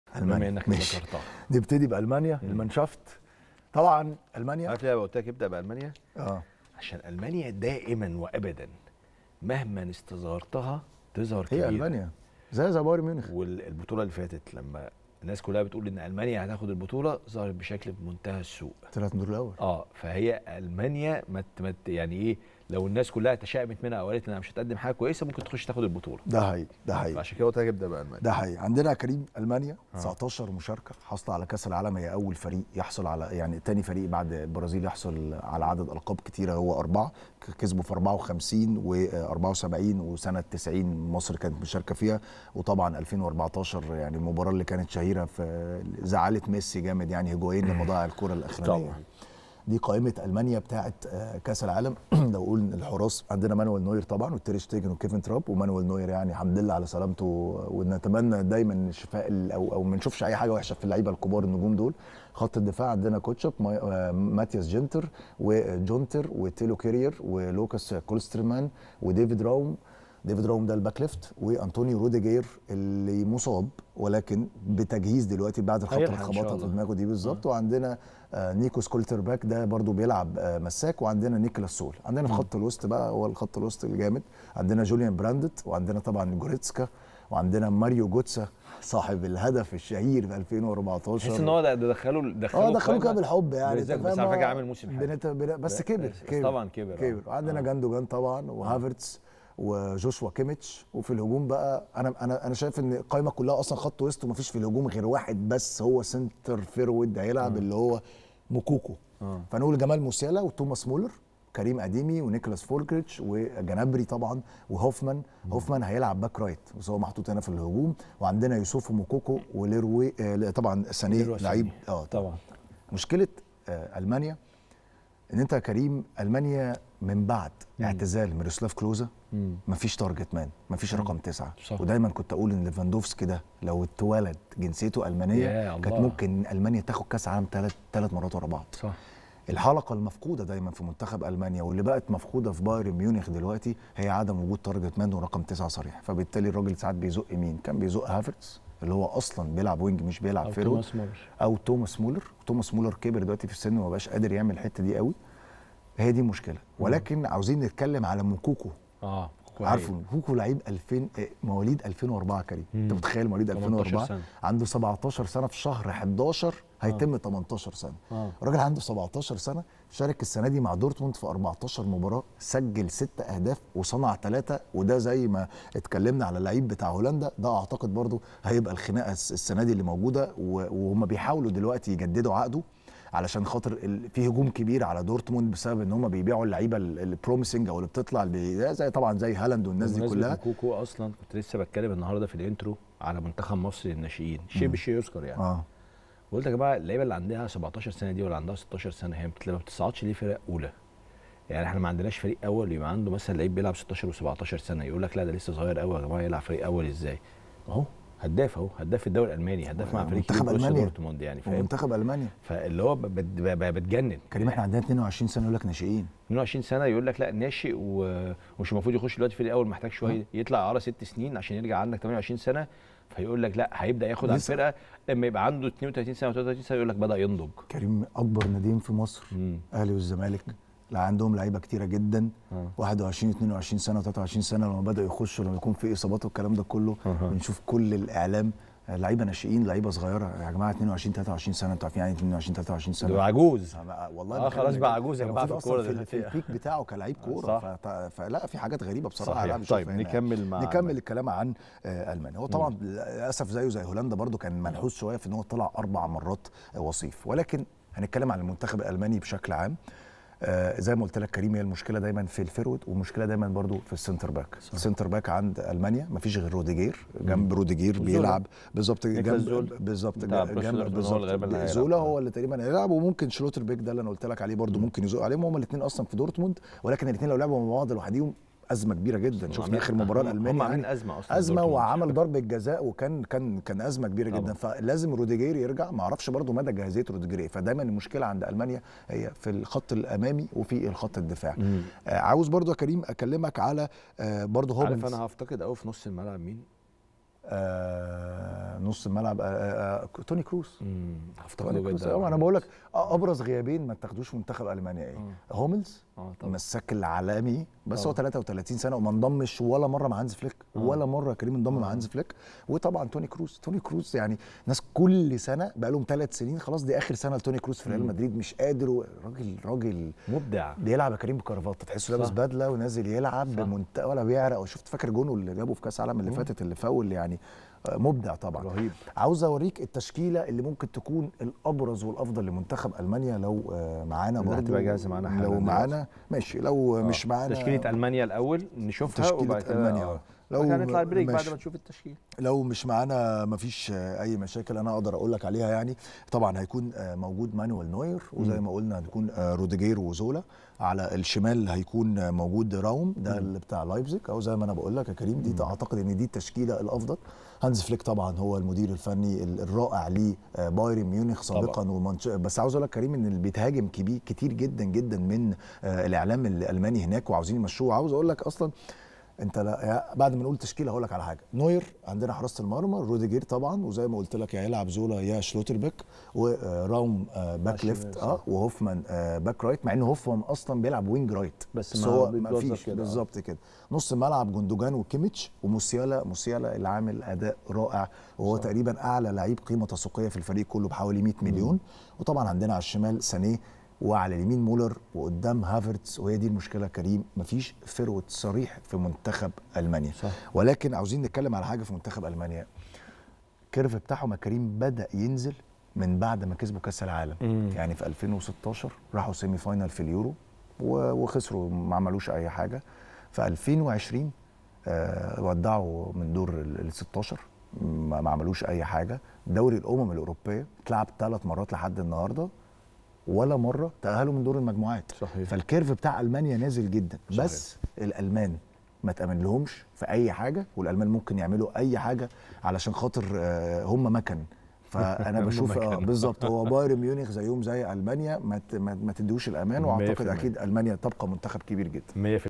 محمود شاكر هيقولك على العيب الوحيد في قائمة ألمانيا .. وهذا اللاعب سيكون مفاجأة الماكينات 🔥💥
المحلل الرياضي محمود شاكر هيقولك على العيب الوحيد في قائمة ألمانيا .. وهذا اللاعب سيكون مفاجأة الماكينات 🔥💥